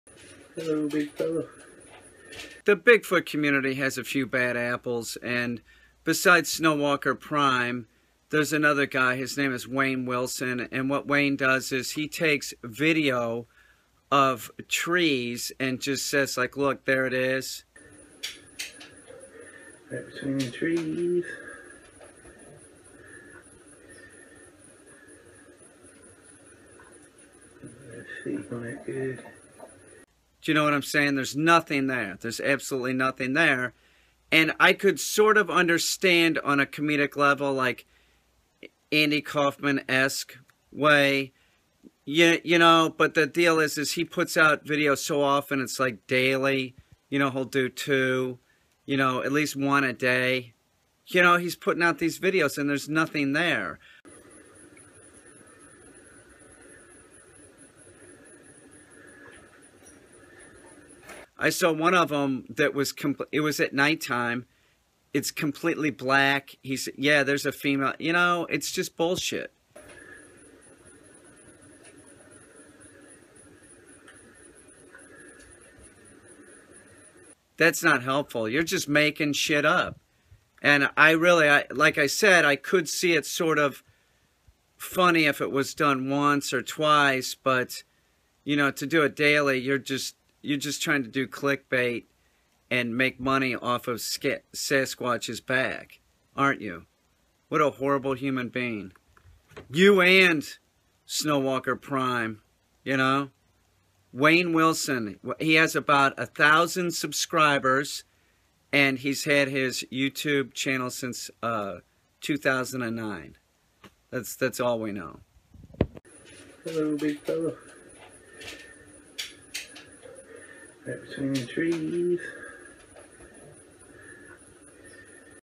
Bigfoot.. Wayne Wilson Hoaxer Sasquatch Researcher
Bigfoot..  Wayne Wilson  Bad Sasquatcher or Savant. Don't watch his B.S.
Readyourdog.com